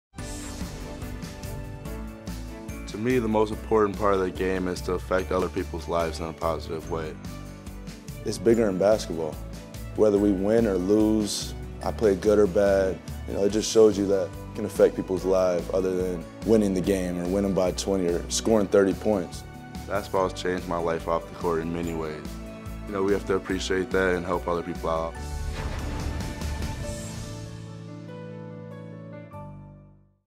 2020-21 Seasonlong NBA Cares Community Assist Award Nominee: Devin Booker
Devin Booker of the Phoenix Suns is one of the nominees for the 2020-21 Seasonlong NBA Cares Community Assist Award presented by Kaiser Permanente. To vote for him or any of the 10 nominees,